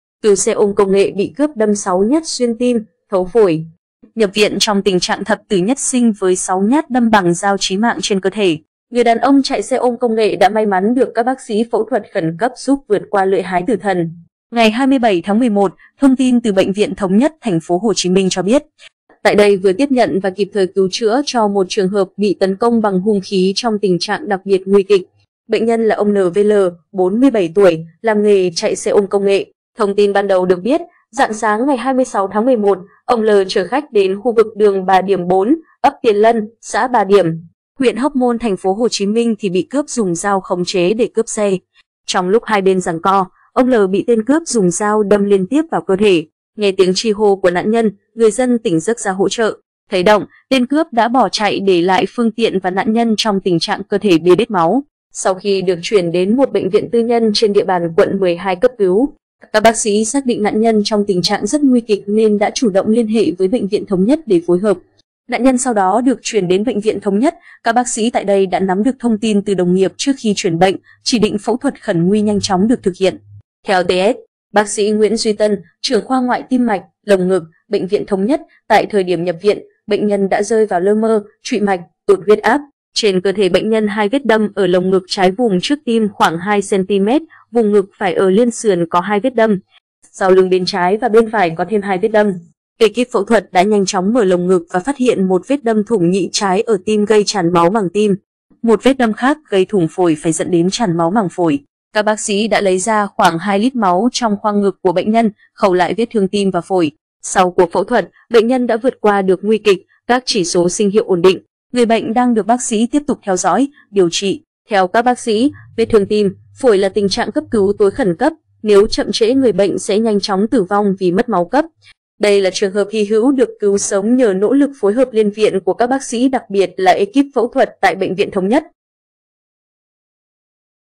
Cứu xe ôm công nghệ bị cướp đâm 6 nhát xuyên tim, thấu phổi
Nhập viện trong tình trạng 'thập tử nhất sinh' với 6 nhát đâm bằng dao chí mạng trên cơ thể, người đàn ông chạy xe ôm công nghệ đã may mắn được các bác sĩ phẫu thuật khẩn cấp giúp vượt qua lưỡi hái tử thần.Thông tin ban đầu được biết, rạng sáng ngày 26/11, ông L. chở khách đến khu vực đường Bà Điểm 4, ấp Tiền Lân, xã Bà Điểm, huyện Hóc Môn TPHCM thì bị cướp dùng dao khống chế để cướp xe. Trong lúc hai bên giằng co, ông L. bị tên cướp dùng dao đâm liên tiếp vào cơ thể. Nghe tiếng tri hô của nạn nhân, người dân tỉnh giấc ra hỗ trợ. Thấy động, tên cướp đã bỏ chạy để lại phương tiện và nạn nhân trong tình trạng cơ thể bê bết máu.
Ngày 27/11, thông tin từ Bệnh viện Thống Nhất TPHCM cho biết, tại đây vừa tiếp nhận và kịp thời cứu chữa cho một trường hợp bị tấn công bằng hung khí trong tình trạng đặc biệt nguy kịch. Bệnh nhân là ông N.V.L. (47 tuổi) làm nghề chạy xe ôm công nghệ.
Theo TS.BS Nguyễn Duy Tân, Trưởng khoa Ngoại Tim mạch - Lồng ngực, Bệnh viện Thống Nhất, tại thời điểm nhập viện, bệnh nhân đã rơi vào lơ mơ, trụy mạch, tụt huyết áp. Trên cơ thể bệnh nhân 2 vết đâm ở lồng ngực trái vùng trước tim khoảng 2cm; vùng ngực phải ở liên sườn có 2 vết đâm; sau lưng bên trái và bên phải có thêm 2 vết đâm.
Các bác sĩ thực hiện cuộc mổ khẩn nguy, kịp thời cứu sinh mạng người bệnh (ảnh: BVCC)
Sau khi được chuyển đến một bệnh viện tư nhân trên địa bàn Quận 12 cấp cứu, các bác sĩ xác định nạn nhân trong tình trạng rất nguy kịch nên đã chủ động liên hệ với Bệnh viện Thống Nhất để phối hợp. Nạn nhân sau đó được chuyển đến Bệnh viện Thống Nhất, các bác sĩ tại đây đã nắm được thông tin từ đồng nghiệp trước khi chuyển bệnh, chỉ định phẫu thuật khẩn nguy nhanh chóng được thực hiện.
Theo các bác sĩ, vết thương tim, phổi là tình trạng cấp cứu tối khẩn cấp, nếu chậm trễ người bệnh sẽ nhanh chóng tử vong vì mất máu cấp. Đây là trường hợp hy hữu được cứu sống nhờ nỗ lực phối hợp liên viện của các bác sĩ đặc biệt là ê kíp phẫu thuật tại Bệnh viện Thống Nhất.
Ê kíp phẫu thuật đã nhanh chóng mở lồng ngực và phát hiện một vết đâm thủng nhĩ trái ở tim gây tràn máu màng tim. Một vết đâm khác gây thủng phổi phải dẫn đến tràn máu màng phổi. Các bác sĩ đã lấy ra khoảng 2 lít máu trong khoang ngực của bệnh nhân, khâu lại vết thương tim và phổi. Sau cuộc phẫu thuật, bệnh nhân đã vượt qua được nguy kịch, các chỉ số sinh hiệu ổn định. Người bệnh đang được bác sĩ tiếp tục theo dõi, điều trị.